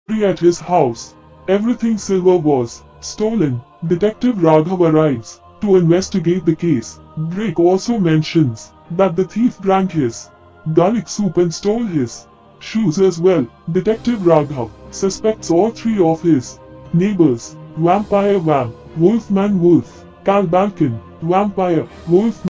the Robbery at Dracula's Palace
Delve into the mystery of the century with "Unveiling Dracula's Robbery"! 🦇🏰 Join us on an epic adventure through the chilling halls of Dracula's ancient palace. 🕵️‍♂️🔍 Discover the truth behind the daring heist that has left the world in awe. 🌟 Historical insights, supernatural tales, and unexpected twists await! 🎬🔔
Akshiti&Shrishti Cartoon Mania channel